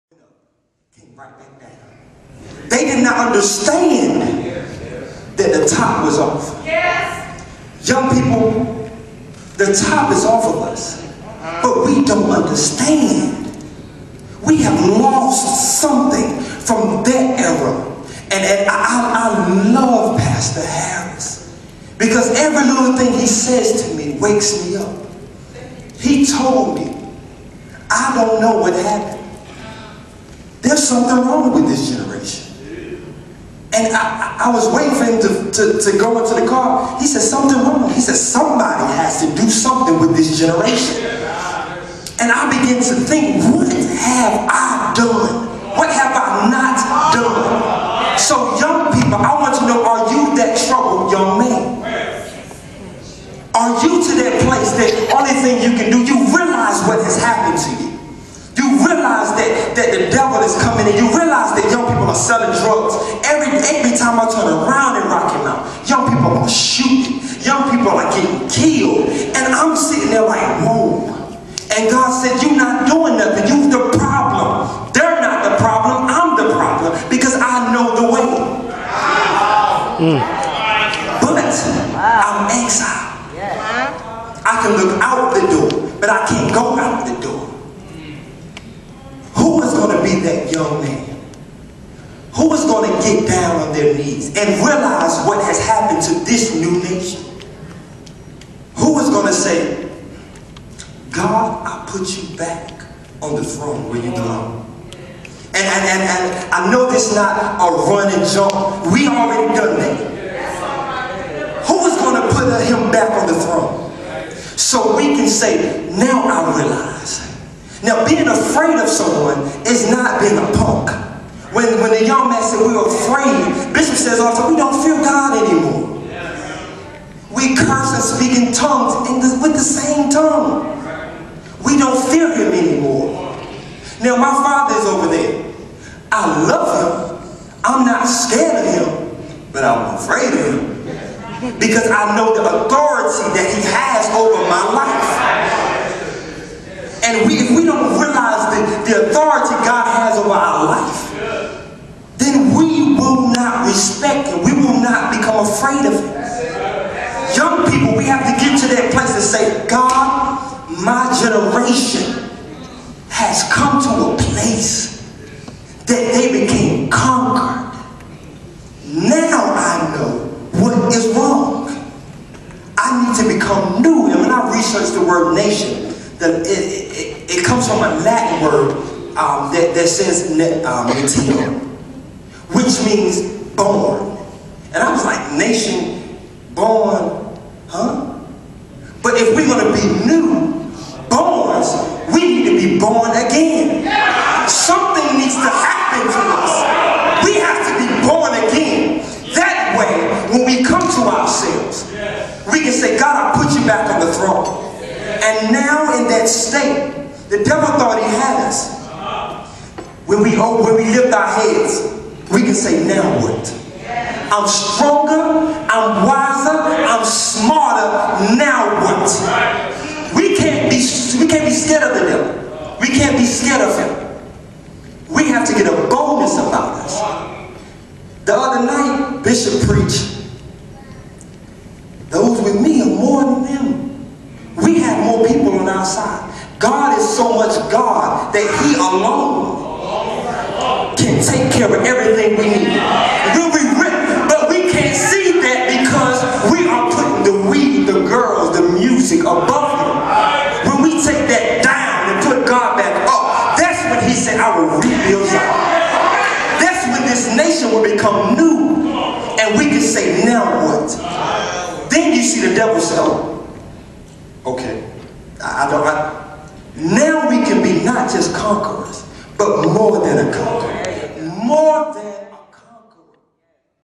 Friday Night Live Spoken Word "Now What"
Youth Explosion: Brother Antoine Brown delivers the mail